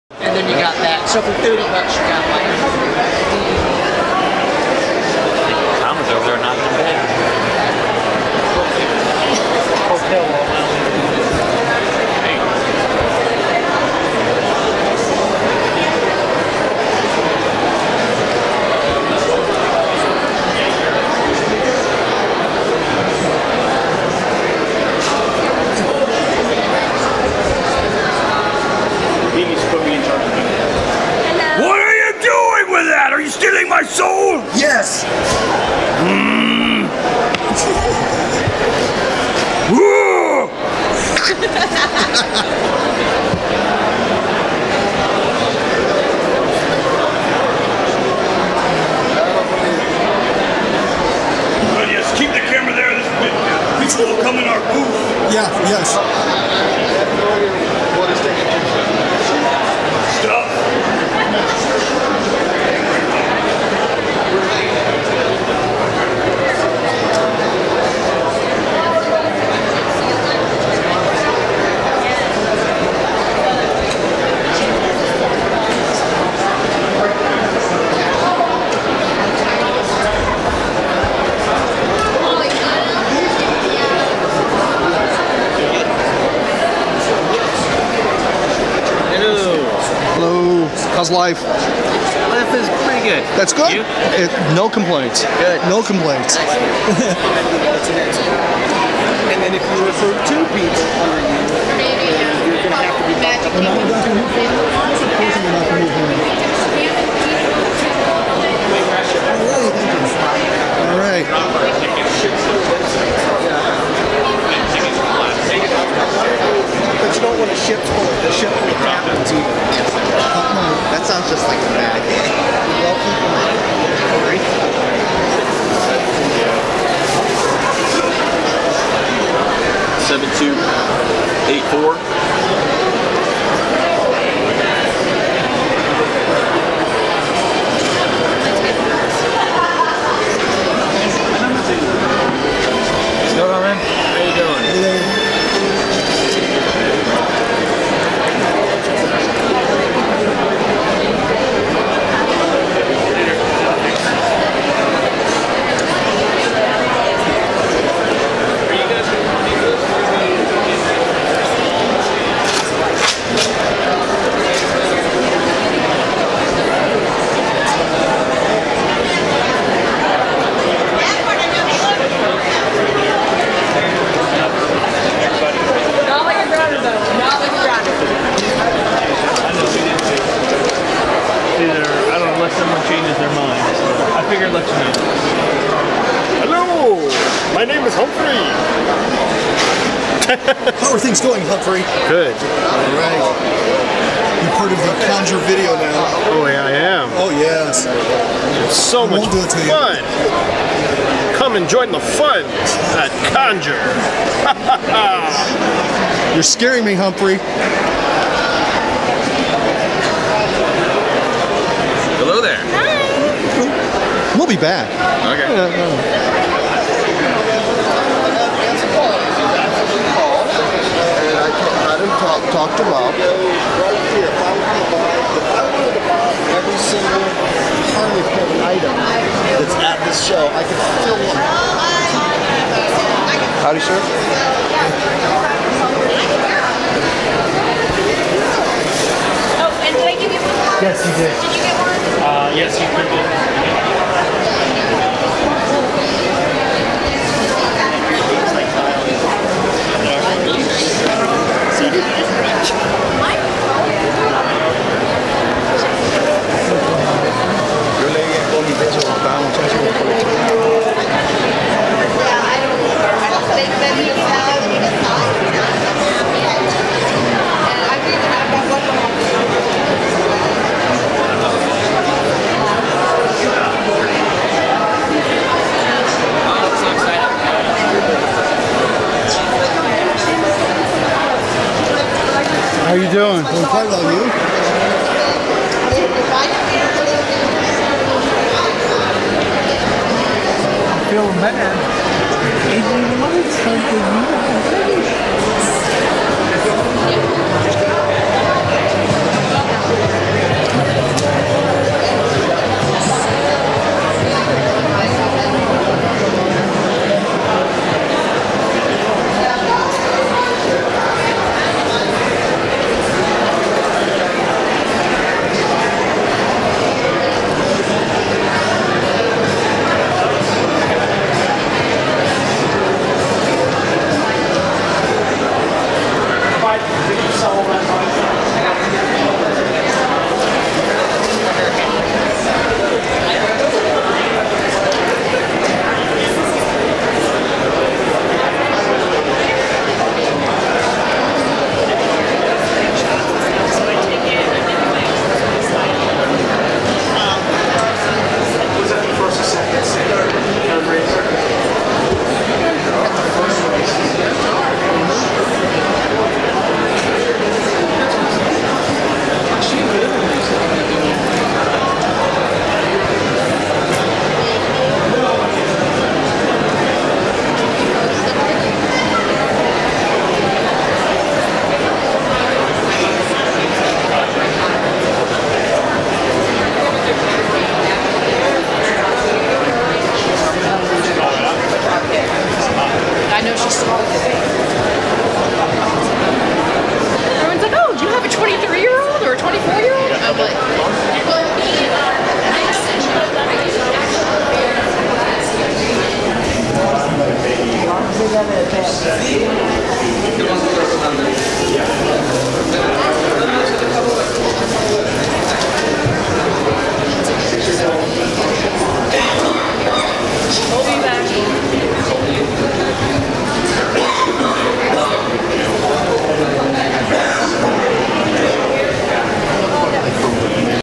Video of Conjure Florida Orlando Convention
Video of Conjure Florida Orlando Convention. Walk thru Noon hour Saturday, March 5, 2016